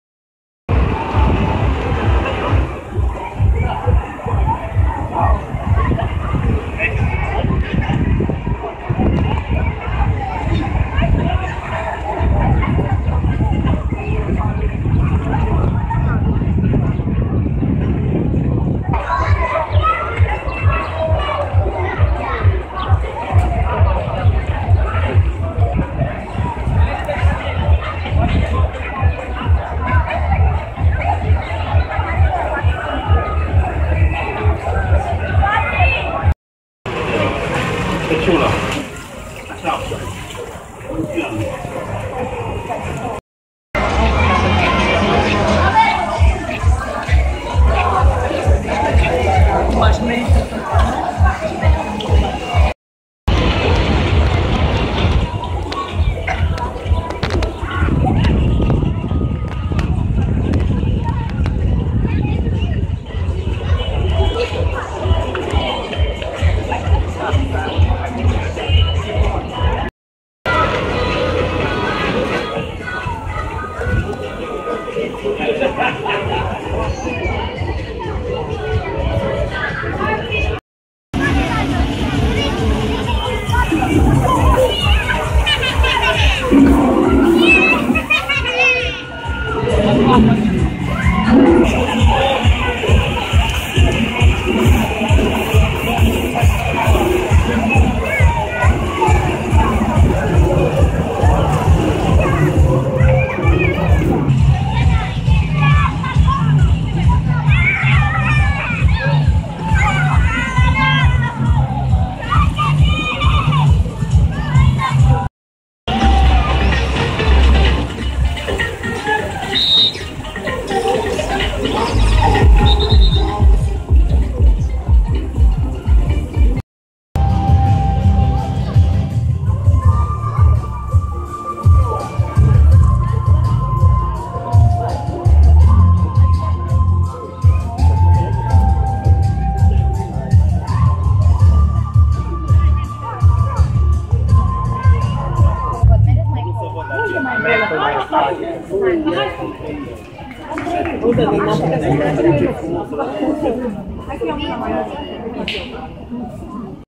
Baile Felix 2016 - Apollo (Thermal Bath)
At only 10 KM from Oradea there is a great place for families -  Baile Felix. Check in the next video the Thermal Bath Apollo one of the Felix attractions and the Waterlilies park.